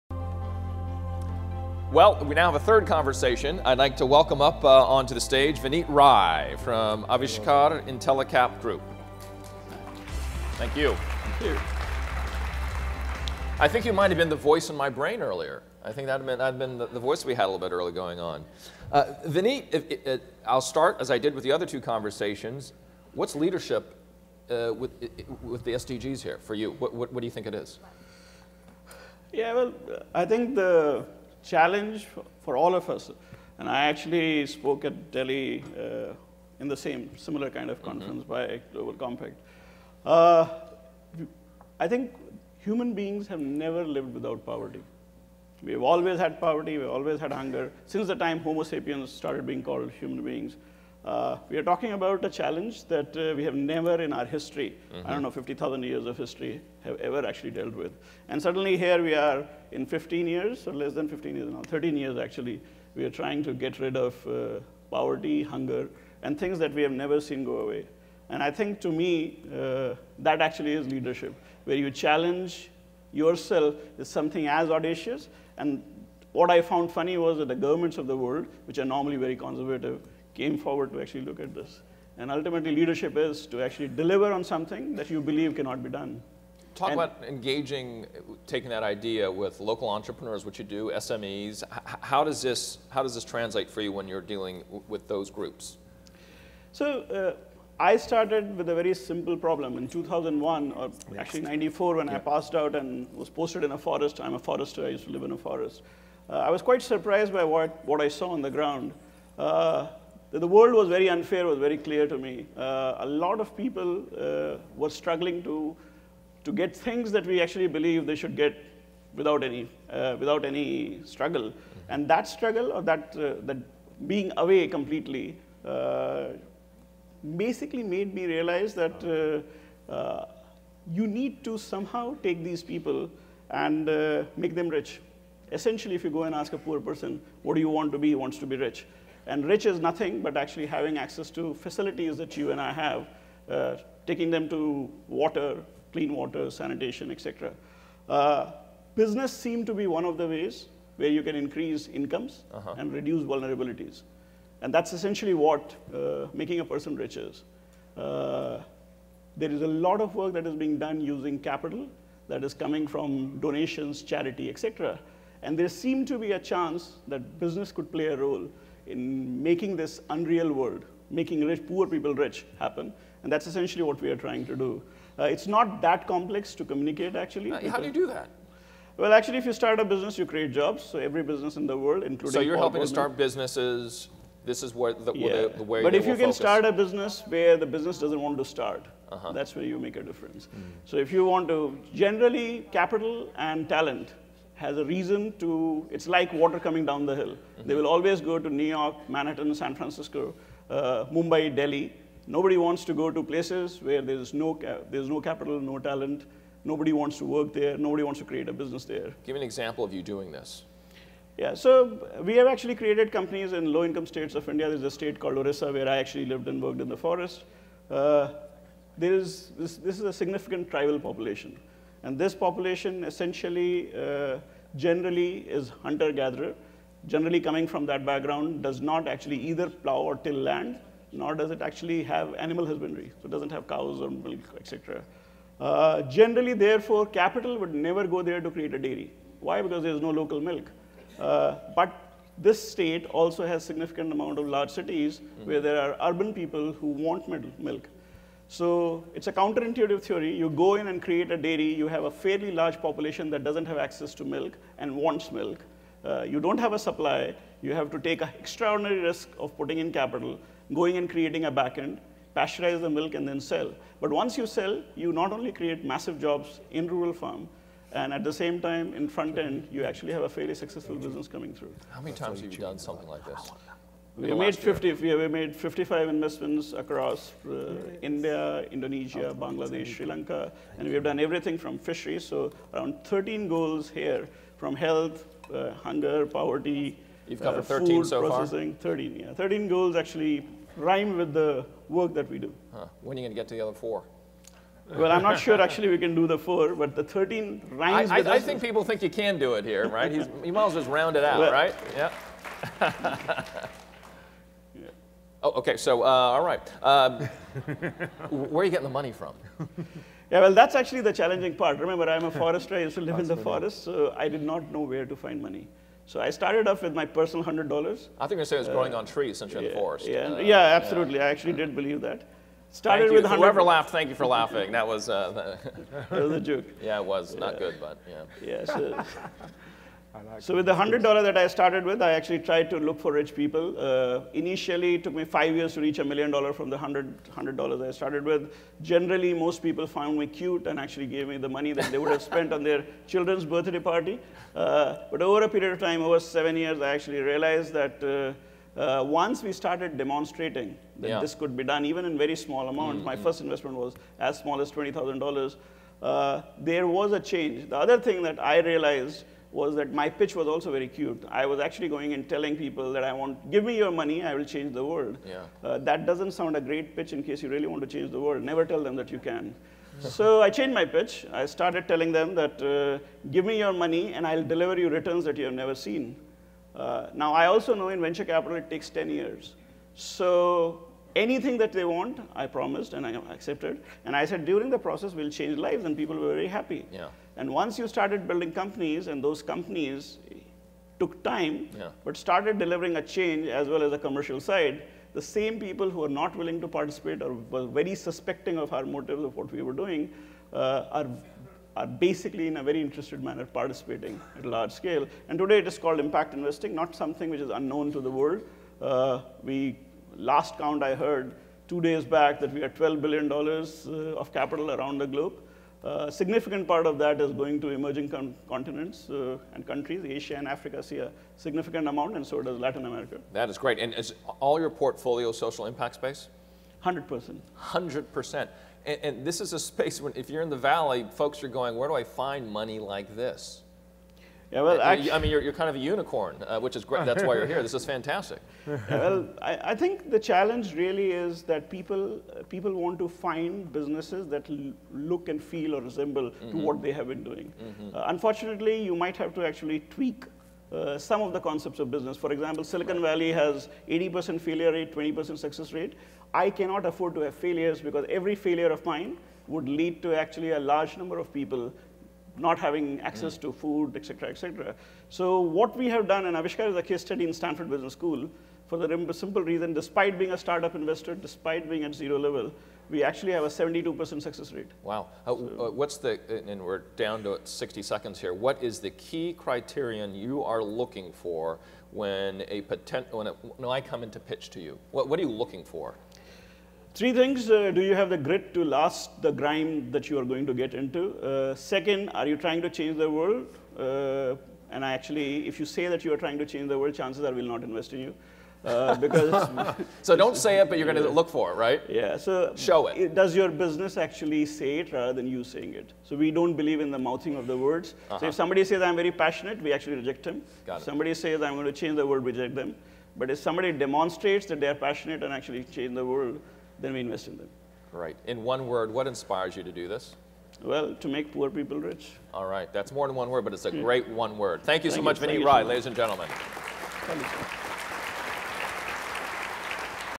Leaders Summit 2017 - Rise to the 2030 Challenge: Vineet Rai
Hear a thought-provoking conversation with industry leaders on challenging the status quo and transforming business to meet the 2030 challenge

Speaker:
Vineet Rai, Founder, Aavishkar-Intellecap Group